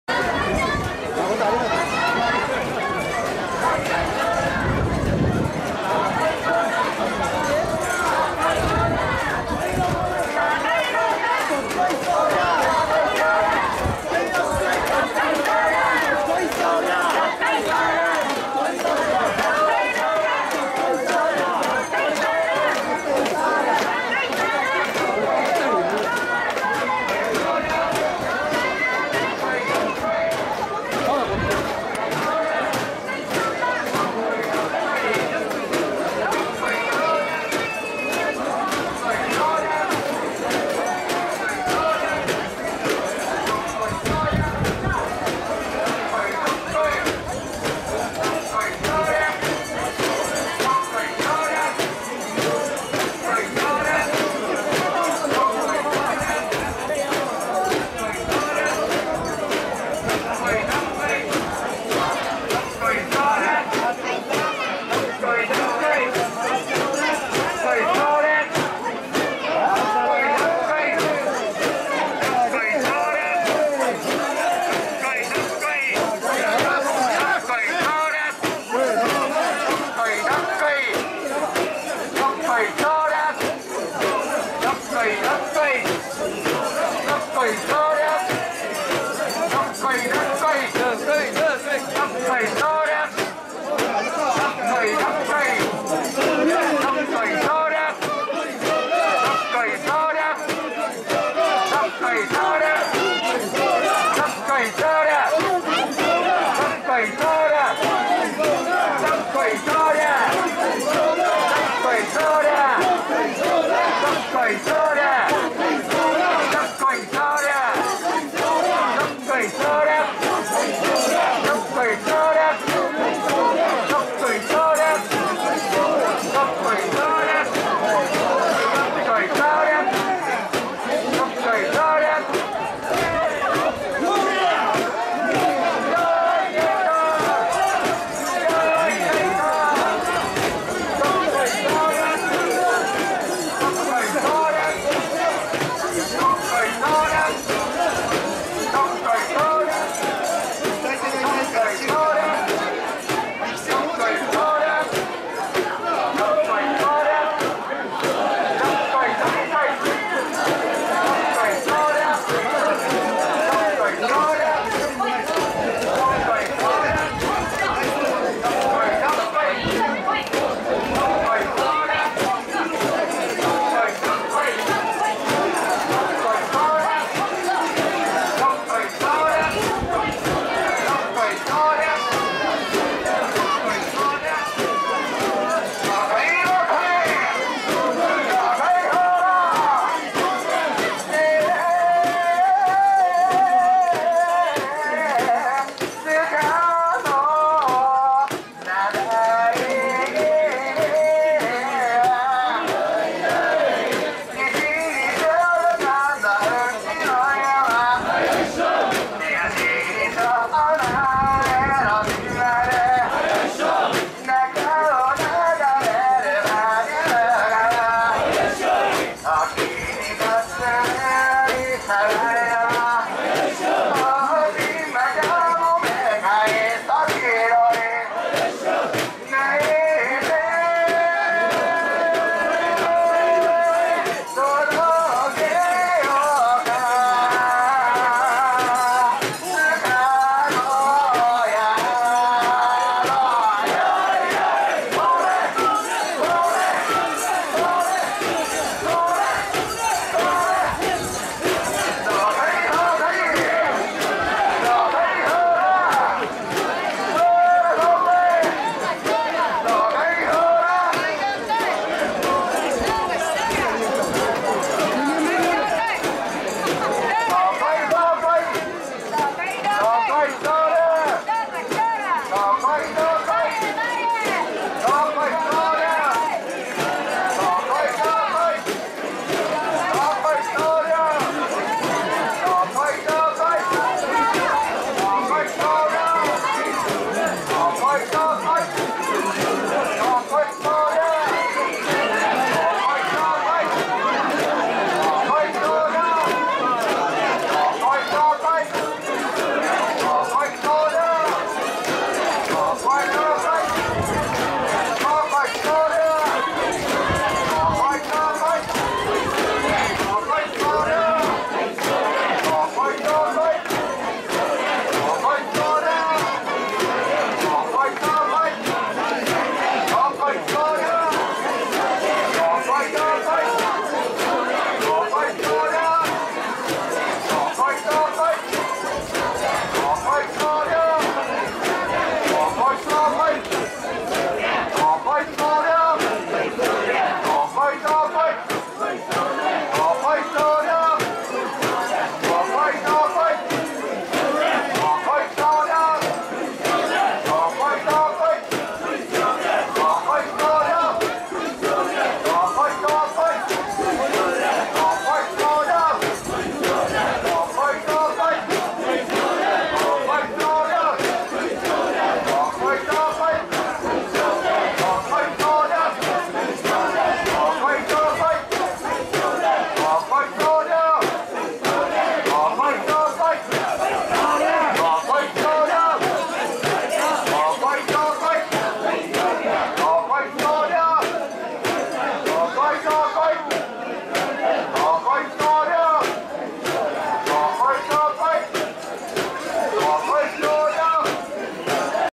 平成27年 平塚 三嶋神社 須賀の夏祭り 本社神輿渡御。
キューバとジャパン 文化交流です。担ぎ手の皆さん　今日は大変暑いの中　お疲れ様でした。平塚三嶋神社の神輿も「相州神輿」や「湘南神輿」と呼ばれ、 ２本の棒だけで神輿を支えて台座の両側に箪笥金具を取り付け、 その金具でリズムをとり、「ドッコイどっこい！」と独特の掛け声で担ぐ。
特大神輿は昭和50年建造。屋根は梨地色で台座は四尺二寸（1.26cm） で胴の部分は龍や獅子の彫刻が金箔に包まれている金粉大神